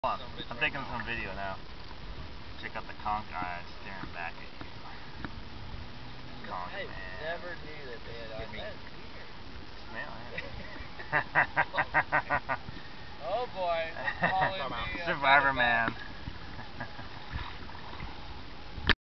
Conch survivorman
Conch out of their shell get eaten alive by a Bahamian survivorman